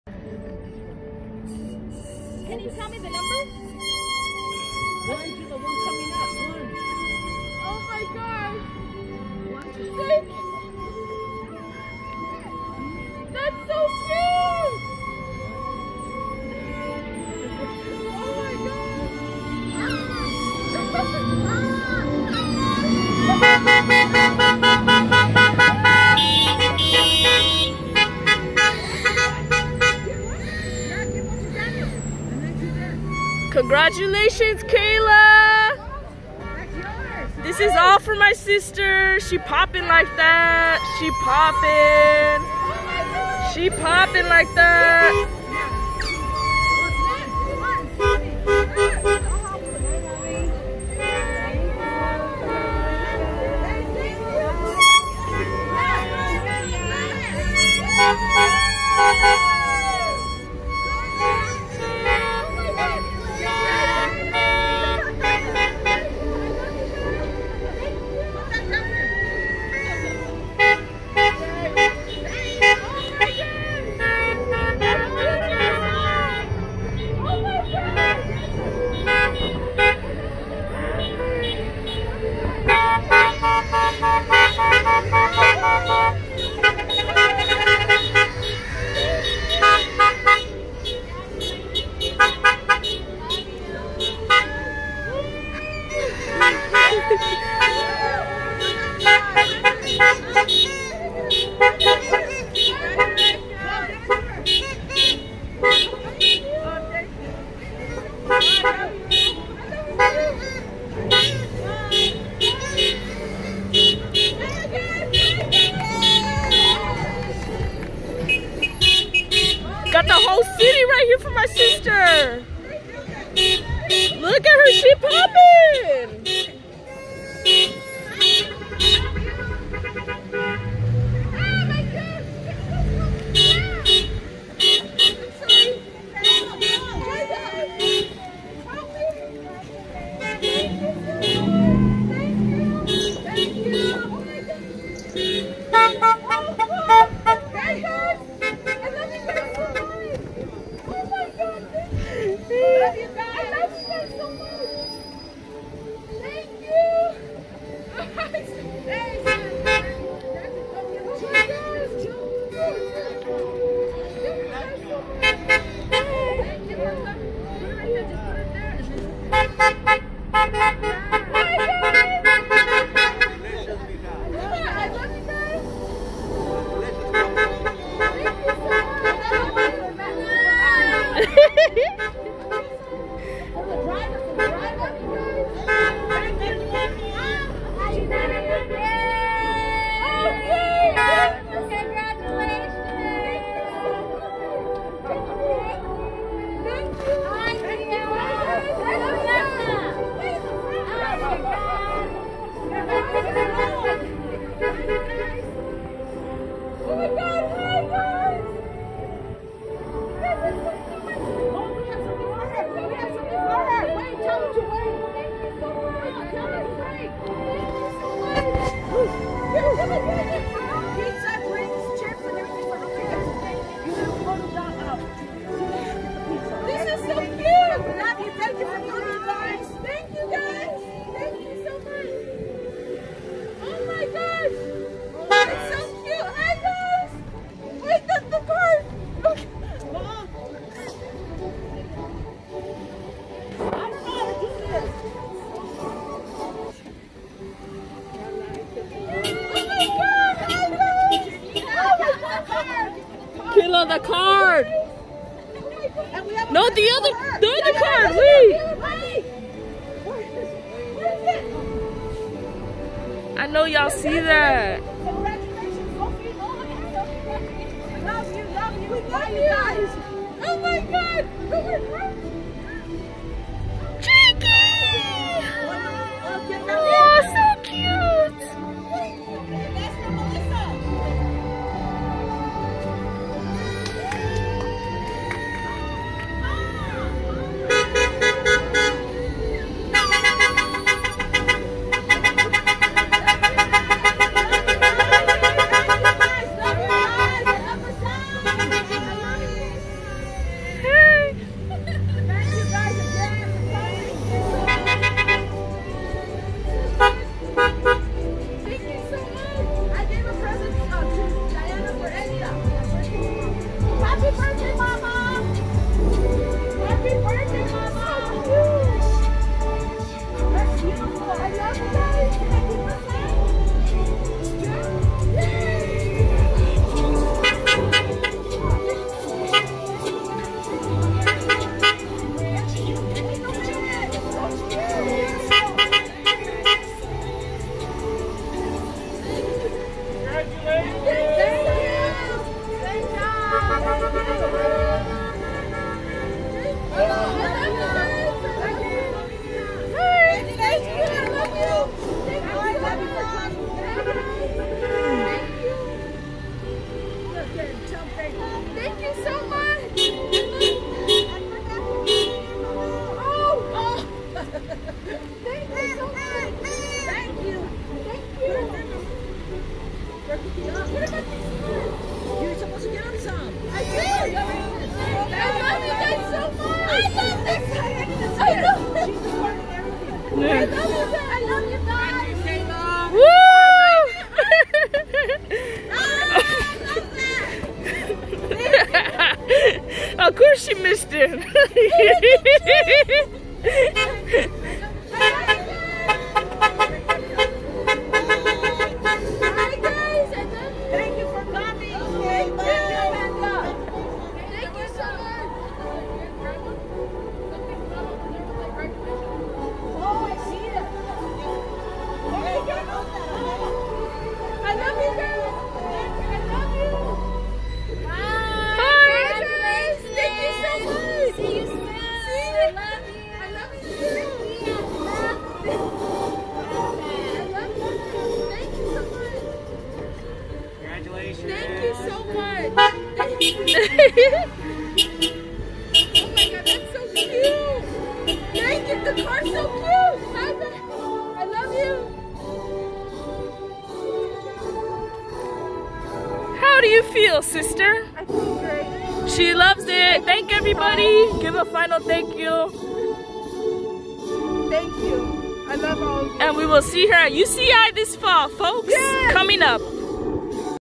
🥳KK's 2020 High School Graduation Parade
Family and friends show their love and support with a social distancing parade for the HS Graduate!